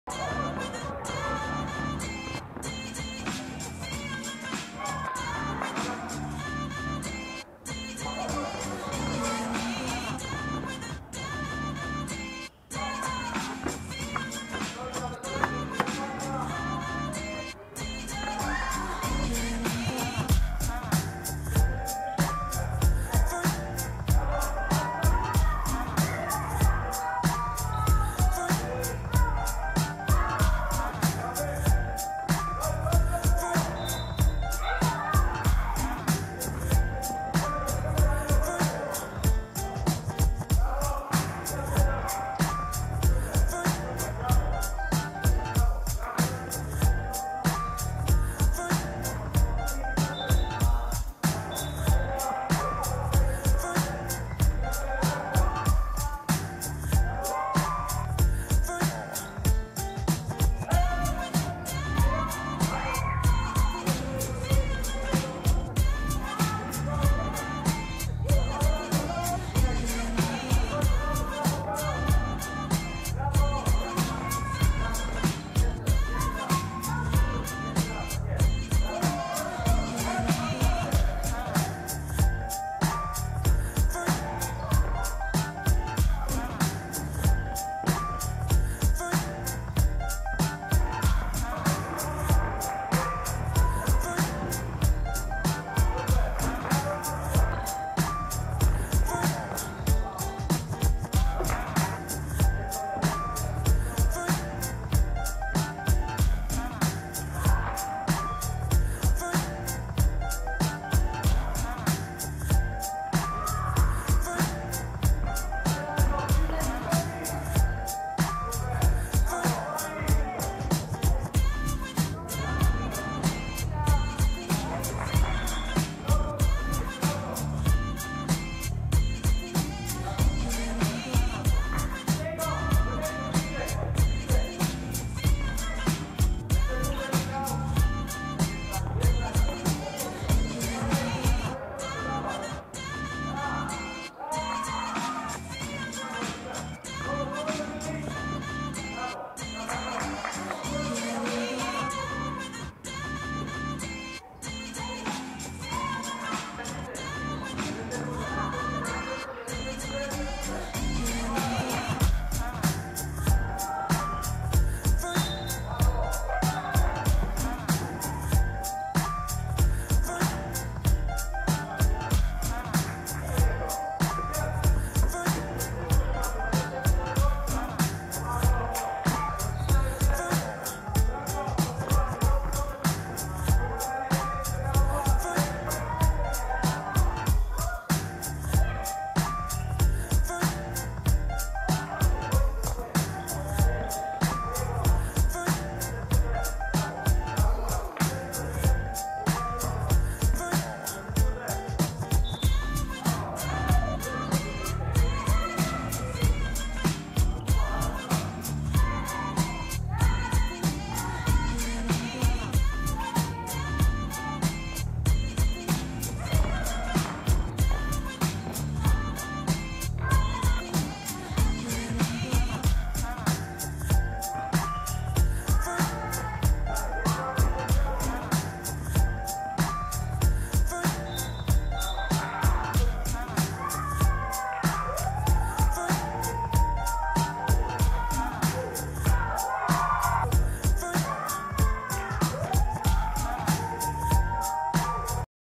04 07 Półfinał Golden State vs Oklahoma – Draft Camp 24 Junior Iława
GOLDEN STATE 83:49 OKLAHOMA
J.Śliwiński 23pkt 7-7ft 5zb 5ast
W.Rutkowski 18pkt 8ast